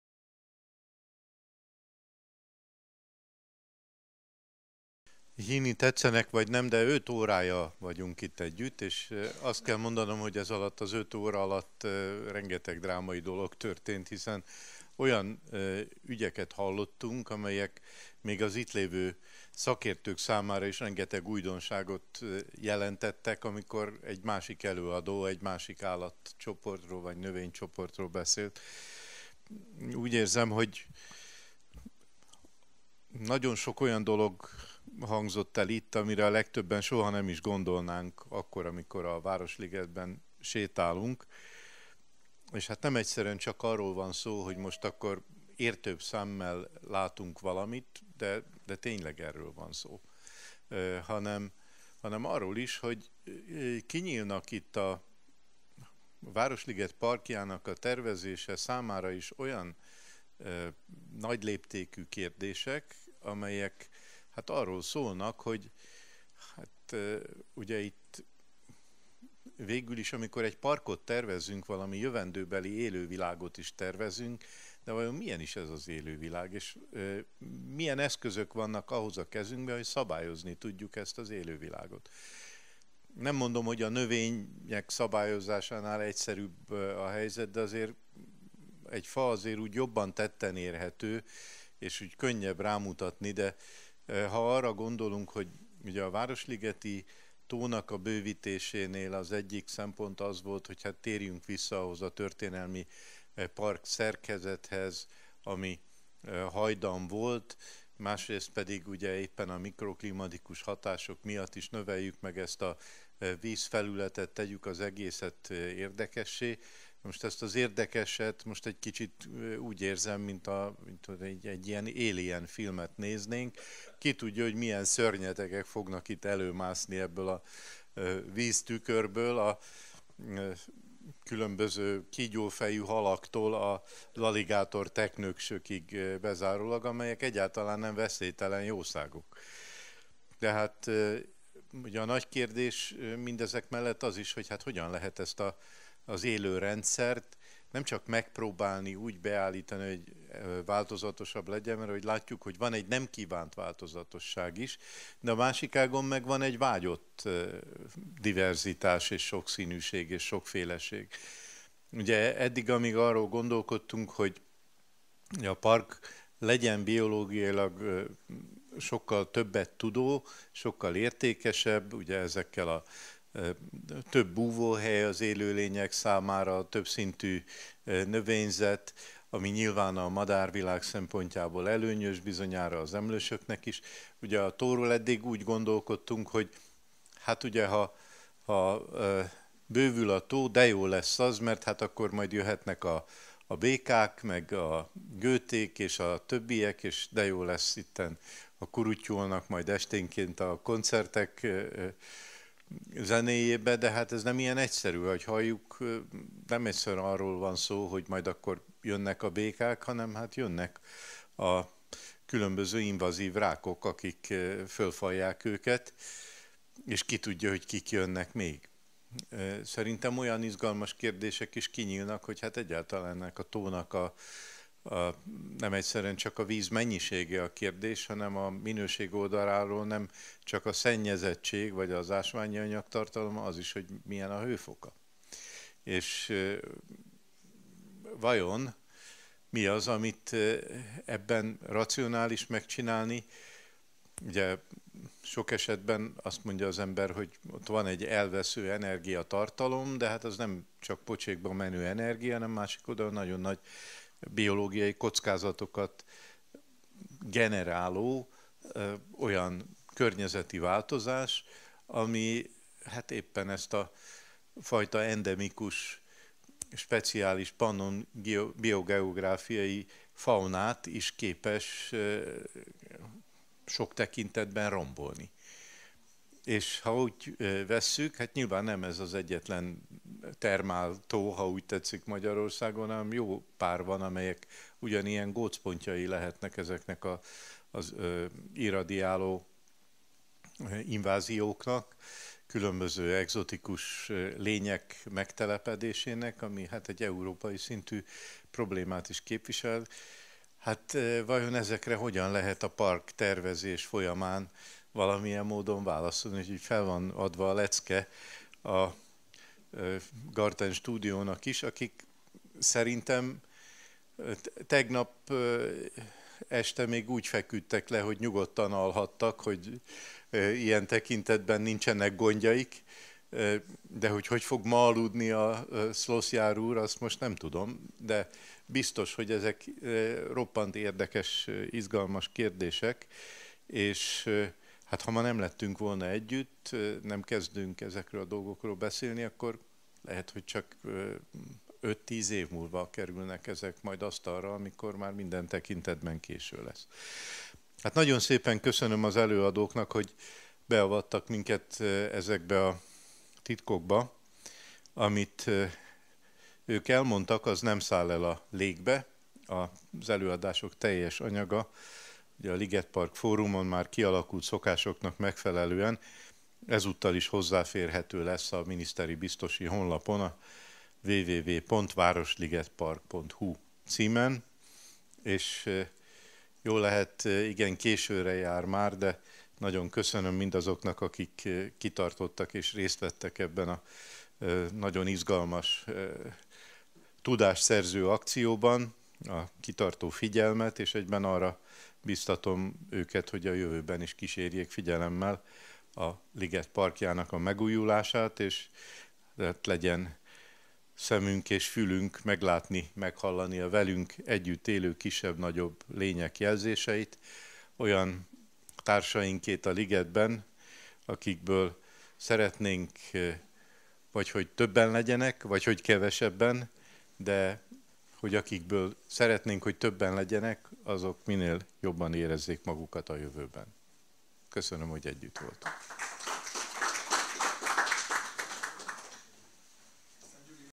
2018.05.10. - Prof. Dr. Persányi Miklós zárszava
Prof. dr. Persányi Miklós, a Liget Budapest projekt részét képező kert- és tájépítészeti feladatok miniszteri biztosa zárszava.